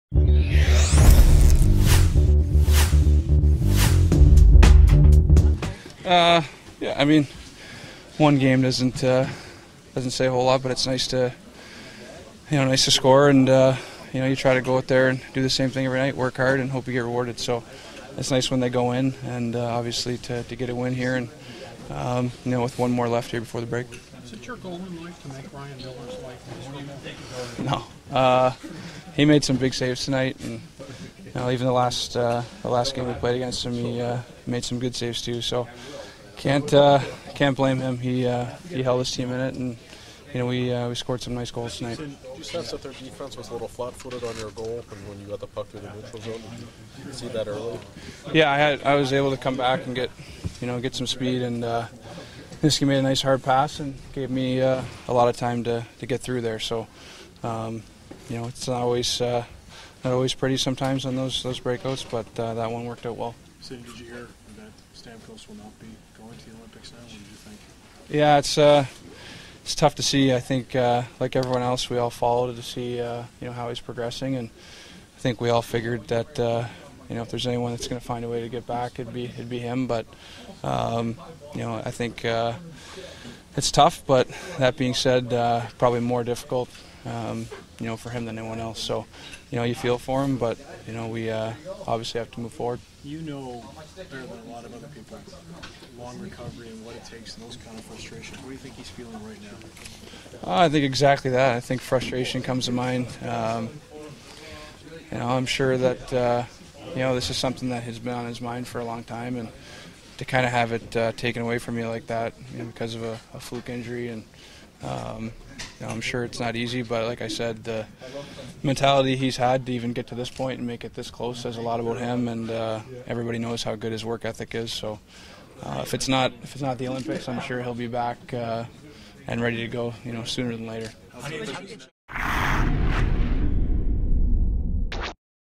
Sidney Crosby: Post Game 2/5/14
First star of the game, Sidney Crosby speaks after the Penguins' 5-1 win over the Buffalo Sabres